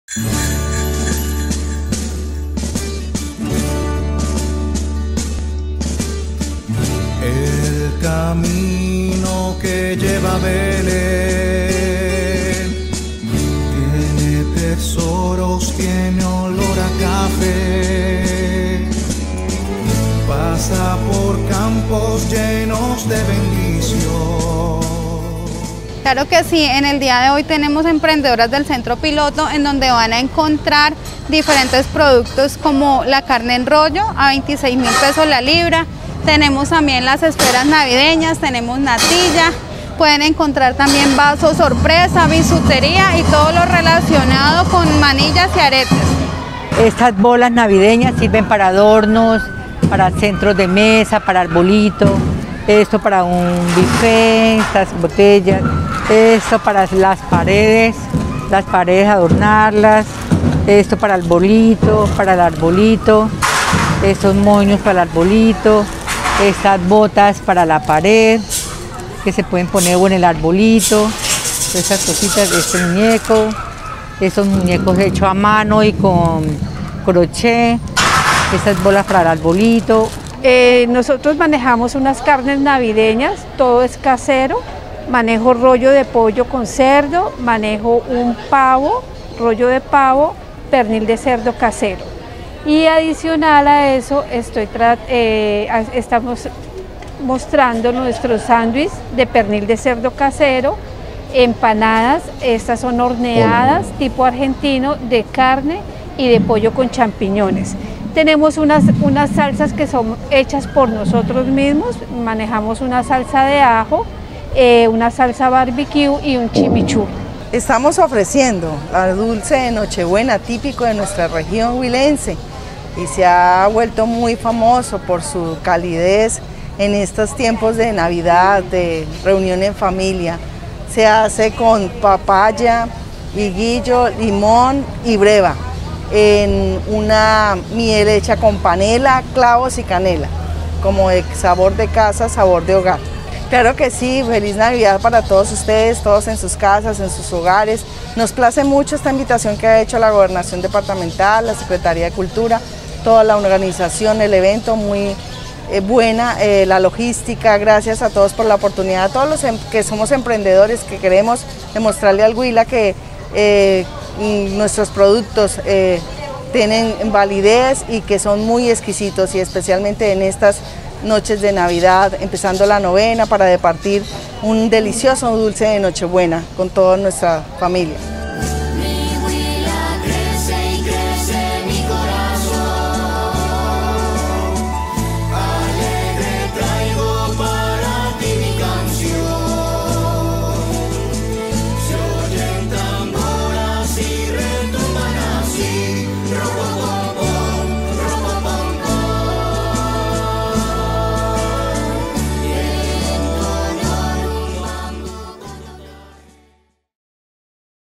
Feria Navideña con enfoque social
La Primera Feria navideña organizada por la Gobernación del Huila a través de la Oficina de Productividad y Competitividad en donde 40 emprendimientos mostraron lo mejor de sus productos relacionados a las festividades decembrinas ha sido el espacio para visibilizar nuevas ideas de negocio con un enfoque social.